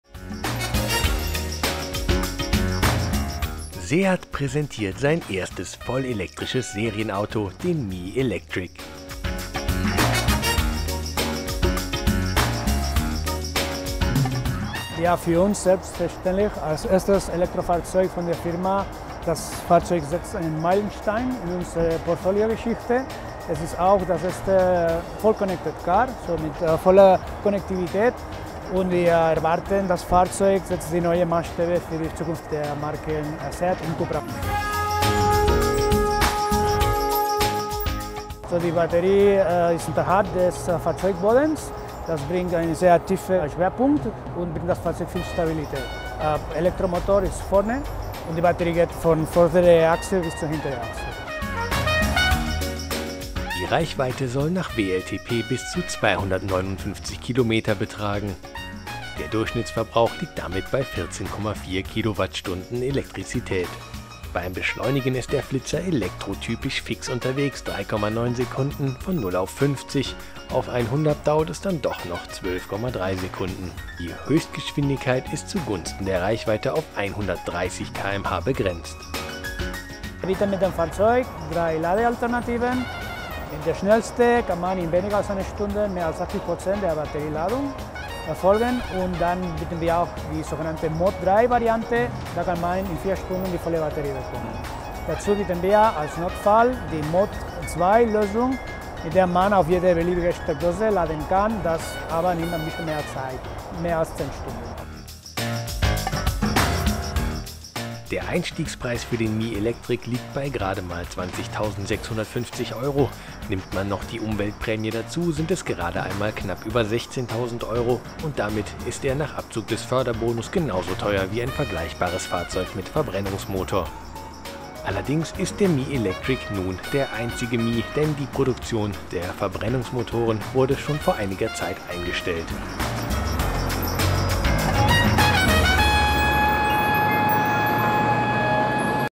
Seat Mii Electric - Gretas Choice
Seat präsentiert sein erstes voll elektrisches Serienauto, den Mii Electric. Das Auto, in dem Greta Thunberg zum Klimagipfel in Madrid gefahren wird.